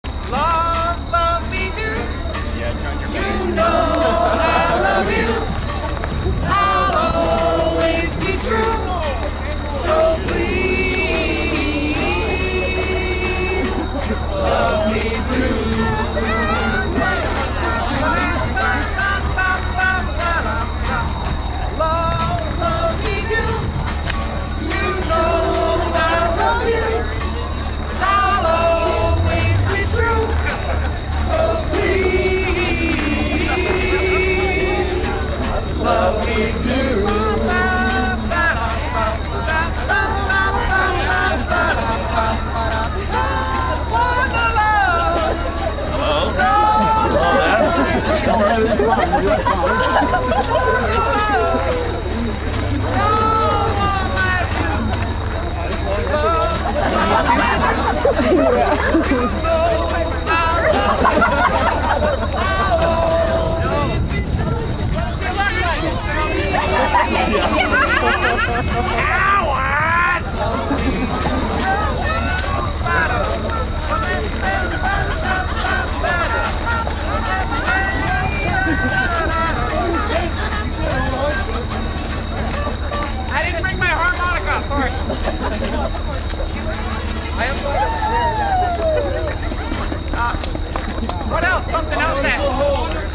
Celebrations Dinner Theatre fire alarm entertainment
A (false) fire alarm went off one night at Celebrations Dinner Theatre in Winnipeg, MB just after dessert was served.  We must have exited out the right door because we were serenaded as we waited for them to clear the area so we could go back in.  Entertainer was  Justin Lacroix. If you notice a bundled up batman, that's Lance Cardinal as Howard.